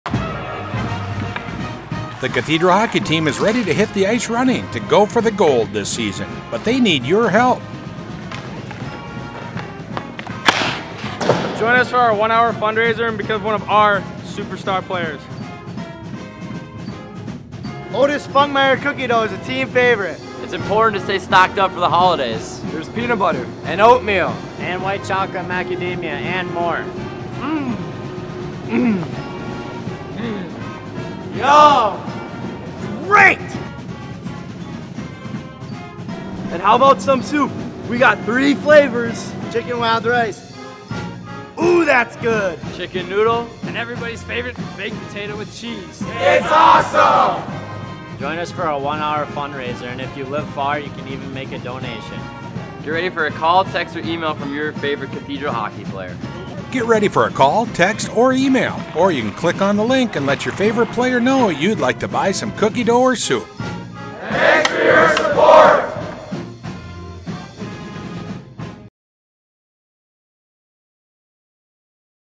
Cathedral Fund 1 Hour Fundraiser 2023
Help support the Cathedral boys hockey team by purchasing some items from the 1 Hour Fundraiser.